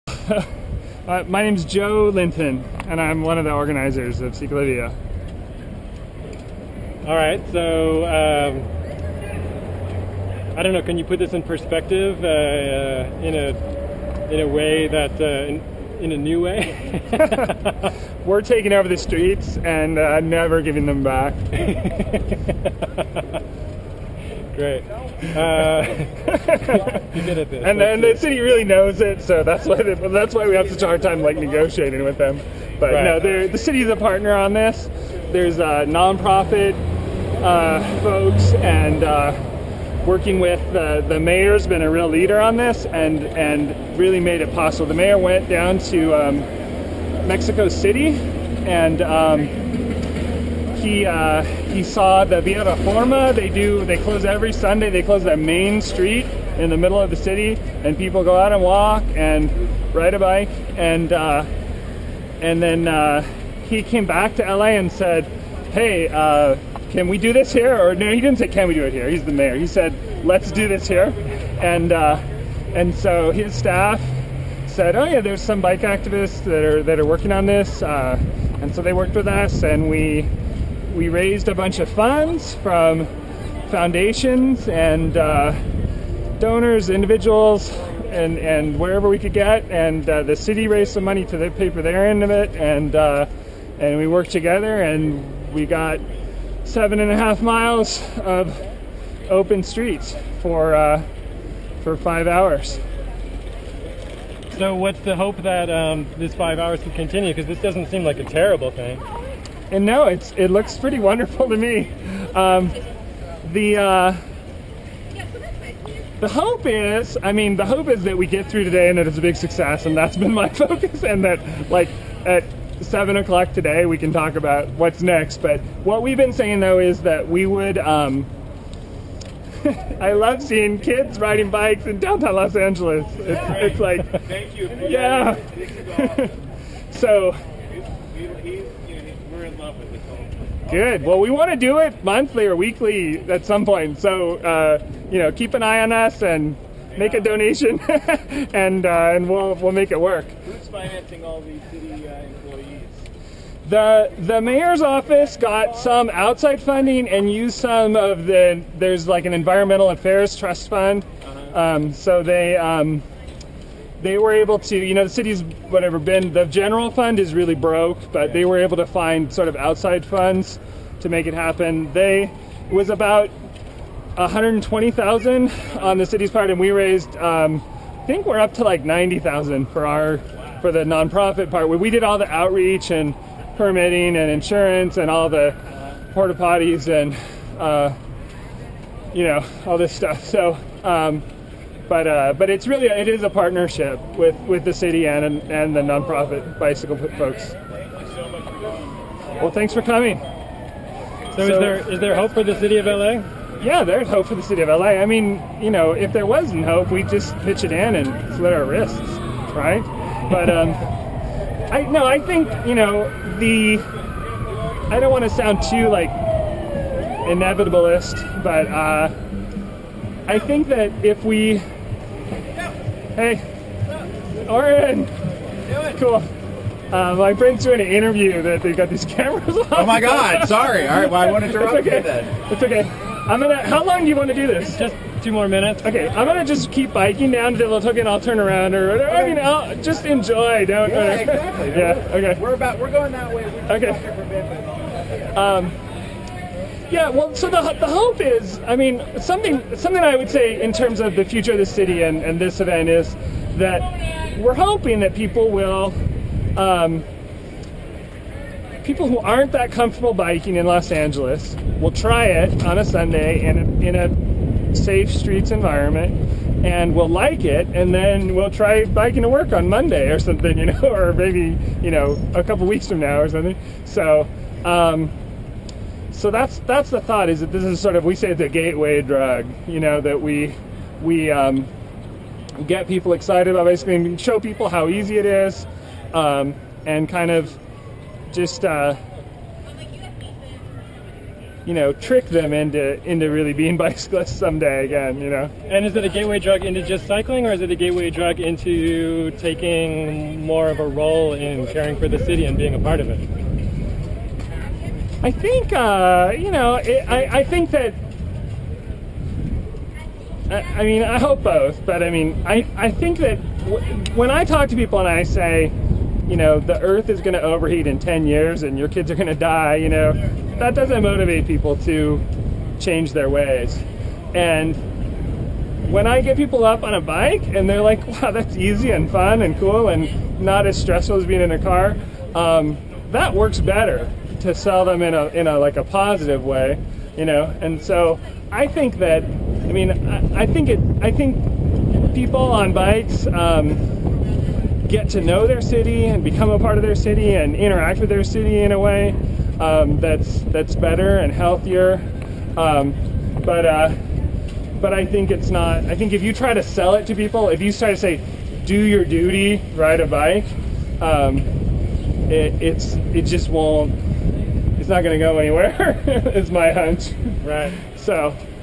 Cycl0ps - Joe Linton at CicLAvia
Cycl0ps - One Eye In Cycling:
Aaron Kuehn interviews Joe Linton (CicLAvia Organizer - Almost Tireless Bike Advocate) in-depth about planning CicLAvia.